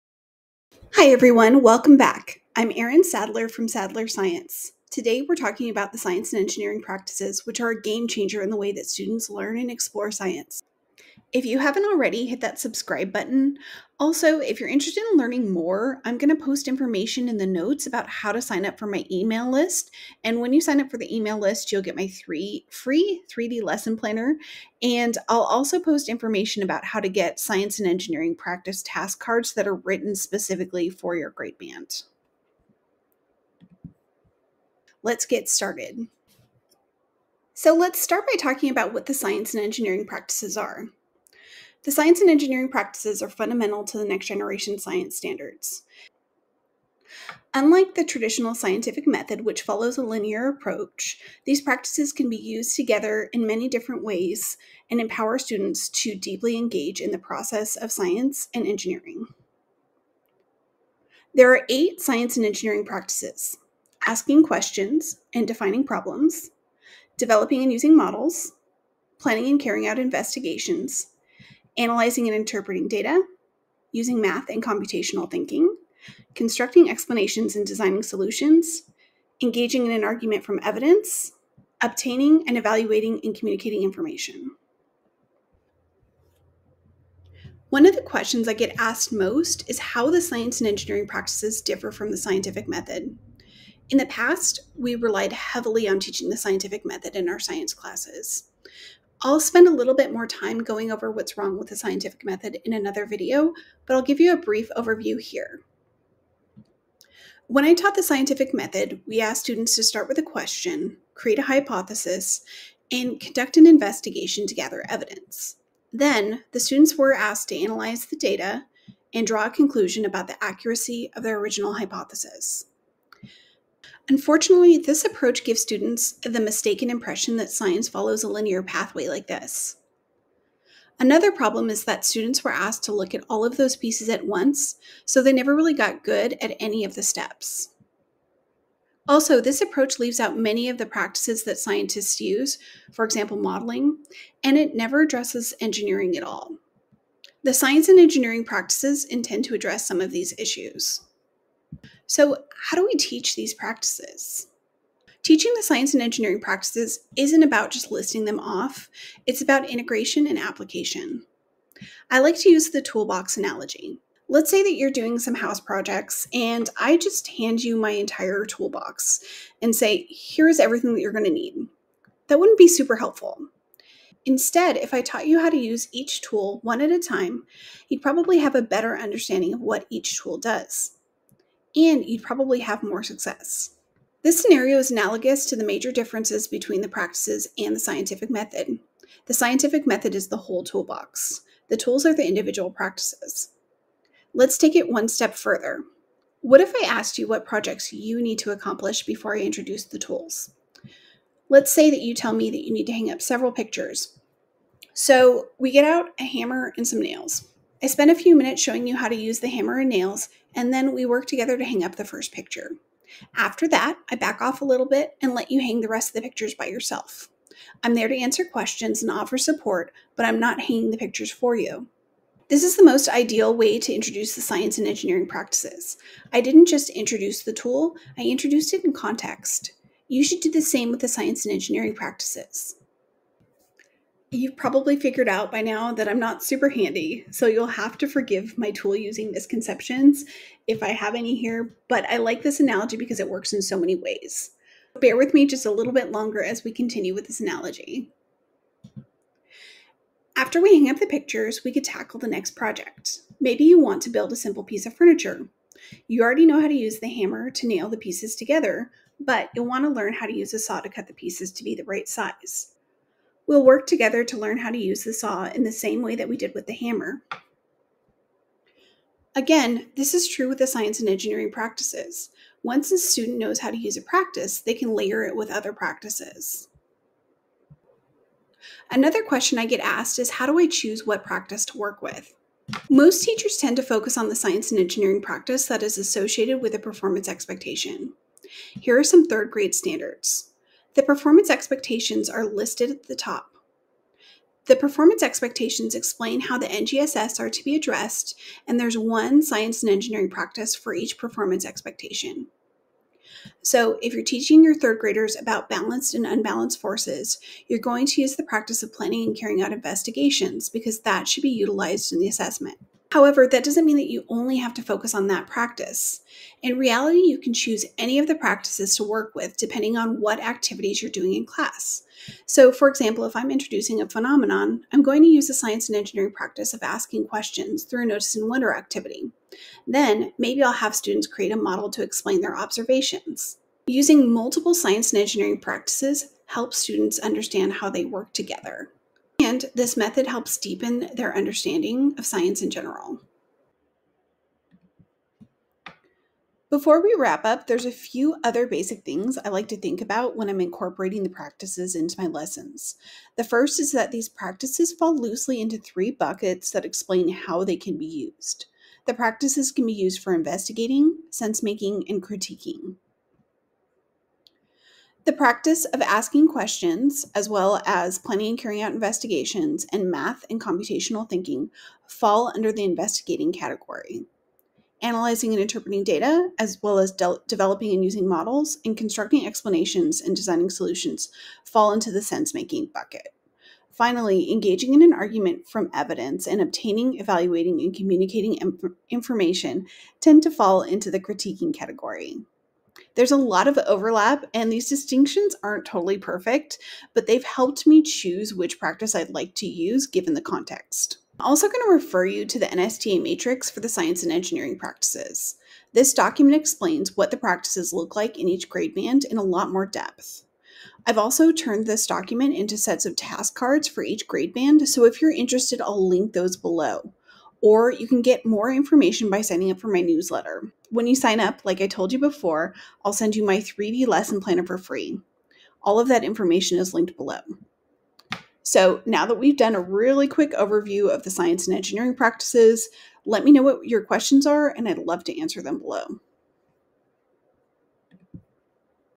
Making Sense of the Science and Engineering Practices
Let's learn about the NGSS Science and Engineering Practices.